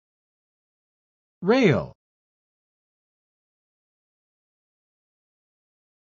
How to Pronounce Rail
This video shows you how to say Rail.